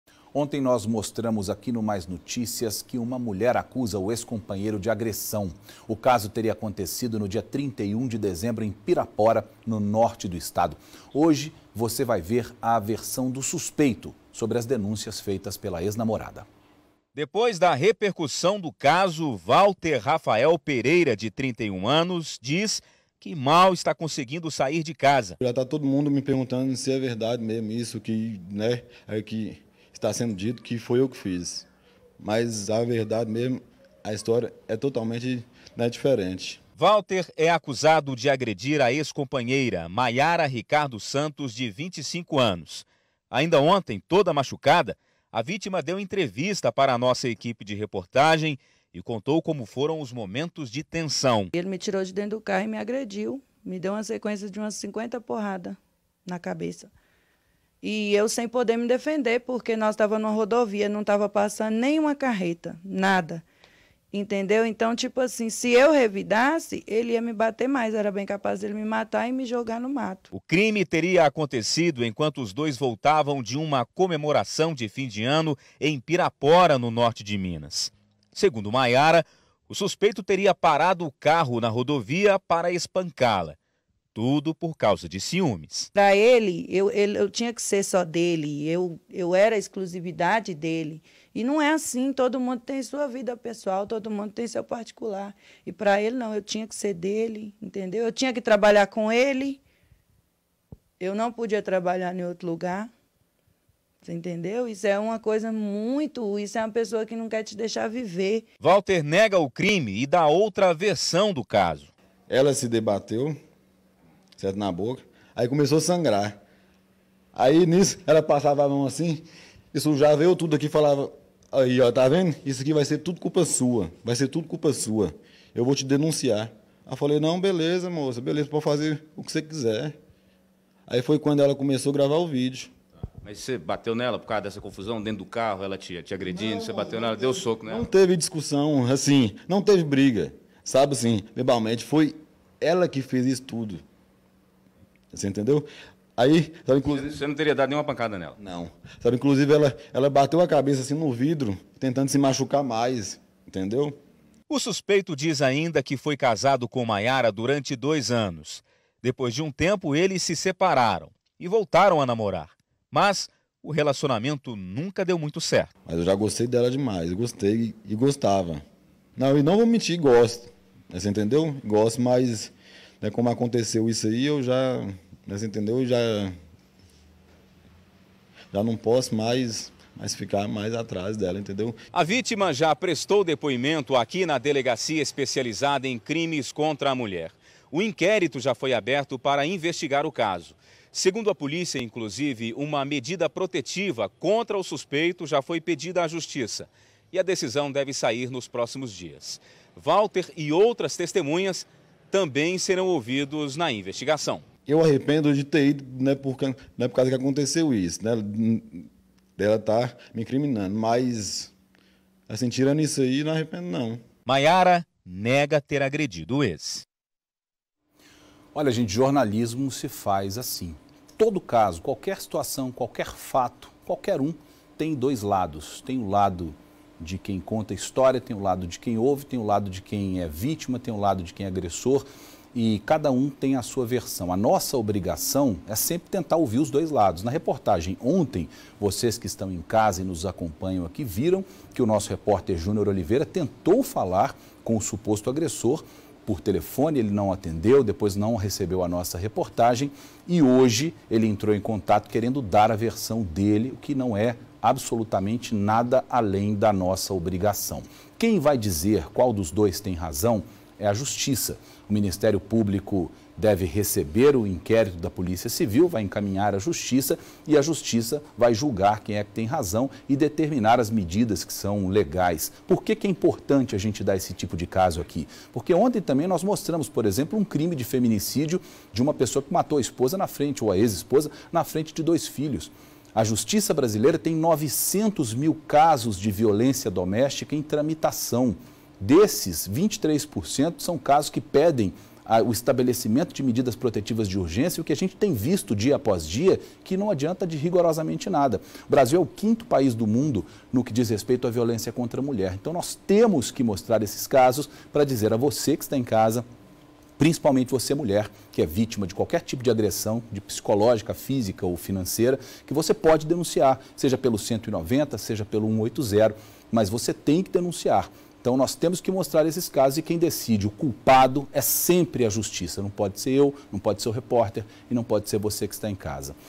Agressão: homem se defende de acusações da ex-companheira
Homem suspeito de agredir a ex-companheira em Pirapora, no Norte de Minas, se defende das acusações.